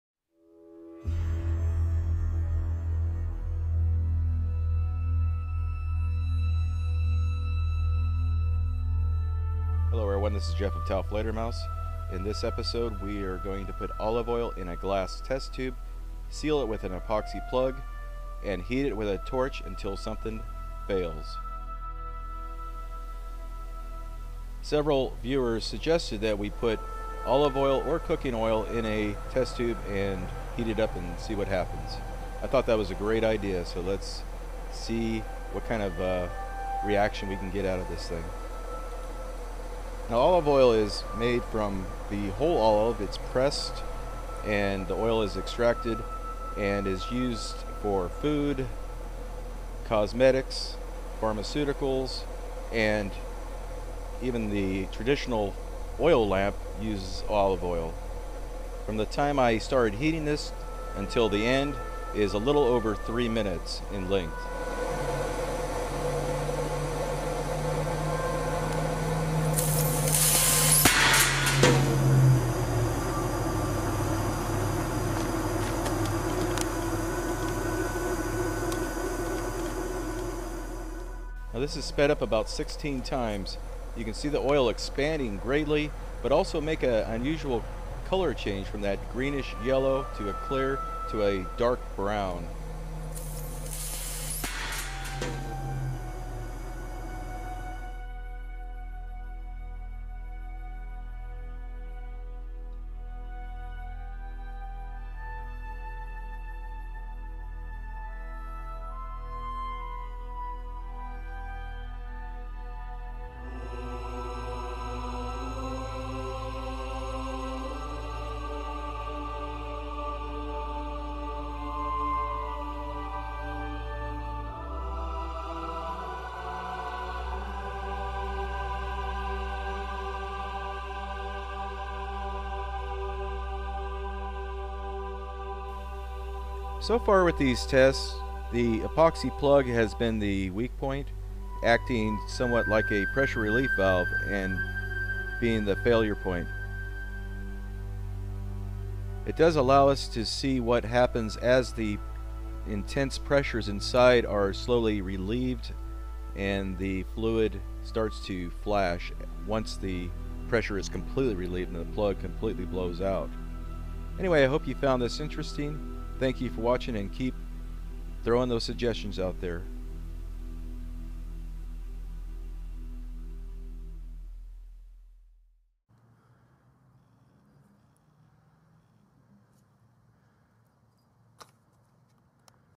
Olive Oil vs GAS TORCH - Stand Clear- this one blew up!
I'm still kicking around a proper, descriptive name for this series.

The test tube fail concept is pretty simple but demonstrates some complex scenarios.
What happens to "x"  when superheated inside a small pressure vessel and put under hundreds of pounds  of pressure?

This episode we put olive oil in a Pyrex test tube,  seal it and heat it with a blow torch.

High speed cameras:  Casio EXF1 Pro's

Common sense dictates that this is dangerous.   Although this is a "small-scale" test,   it is dangerous to some degree.   We are not trying to make a bomb,   but a pressure vessel with a  controlled-release of pressure to demonstrate pressure/temperature relationships.

-Keep suggesting!   Even if you posted an idea on the last video,  re-post the idea here as I will probably scan these comments for the next idea!  It is very hard to keep track of the ideas,   so that will help me.

(Sorry if I missed anyone!)
Suggested by: AlucardsDarkestNight,  m4xwellmurd3r,  Glenn Simpkins,  A Bush,  Francis Borek,  tapper88,  worldwidepaperchaser,  wazup862,  Igor Giganski.